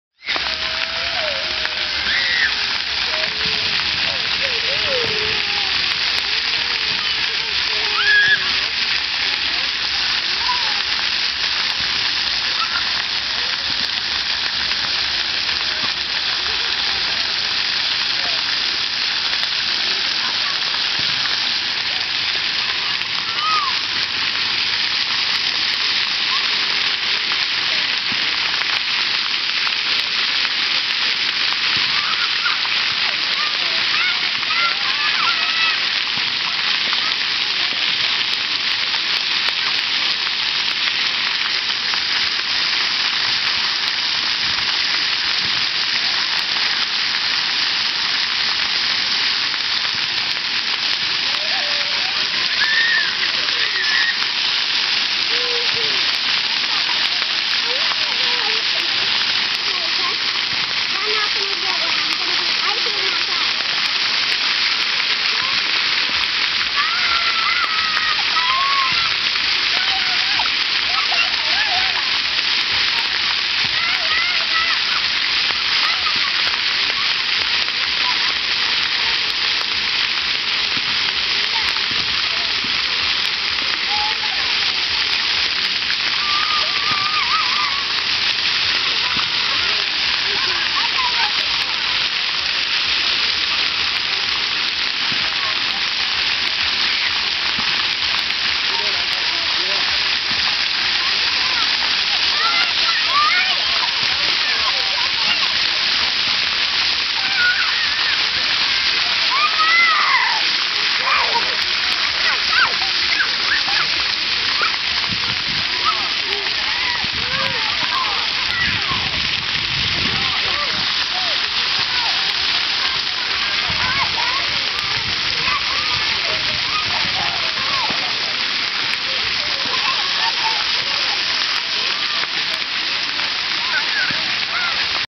Bolling AFB Splashground
Jadyn & Trey playing at the splashground on Independence Day 2011.